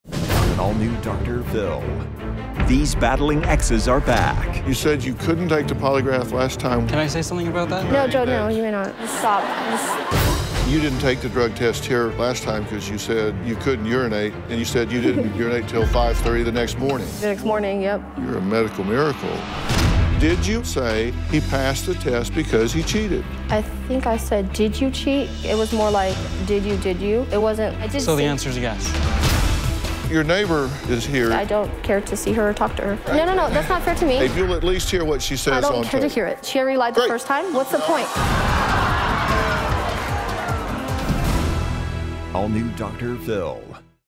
Sandra And Joey Return: Will Sandra Finally Take The Polygraph And Drug Test?
MONDAY, MAY 23: Sandra and Joey, exes in a bitter custody battle with accusations of physical and sexual abuse toward their daughters, return to the show. Will Sandra take a polygraph to help prove she has not coached her daughters to make up allegations against their father? And, what causes her to storm off stage? Plus, on Tuesday, will Sandra return?